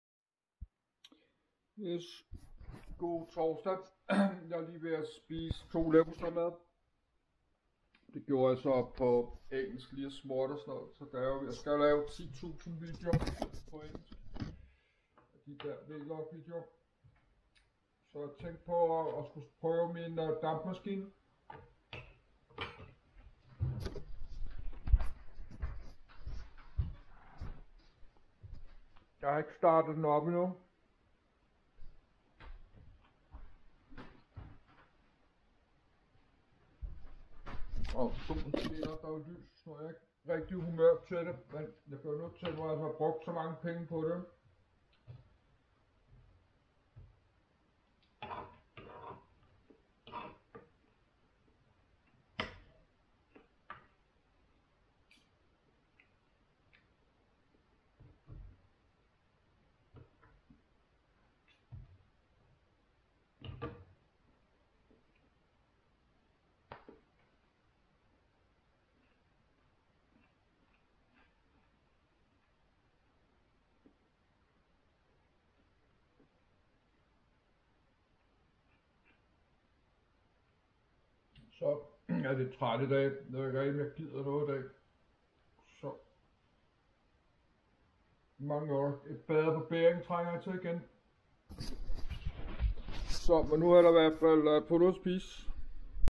MongoTV_11343 - Min Frokost - RUGBRØD Med LEVERPOSTEJ og RISTET LØG
God TORSDAG. Jeg spiser lige 2 stykker rugbrød, med leverpostej. Jeg er stadig lidtr søvndrukken oig træt. Men klokken den er 12.15 snart. God Påske 2023.